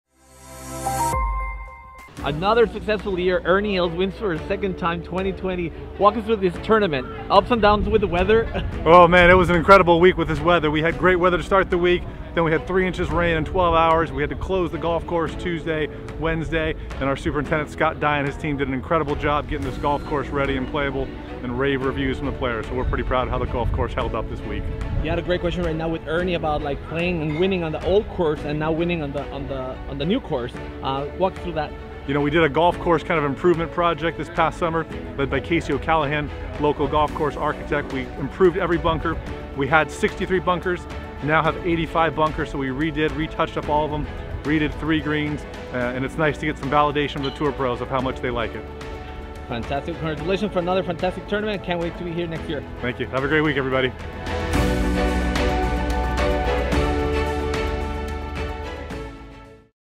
ROBIN SHELTON | 2023 HOAG CLASSIC CHAMPION | INTERVIEW | G SOCIAL NOW
Our very own @gabo_style got to talk with Robin Shelton, NBCC General Manager after the final round at the 2023 Hoag Classic in Newport Beach Country Club.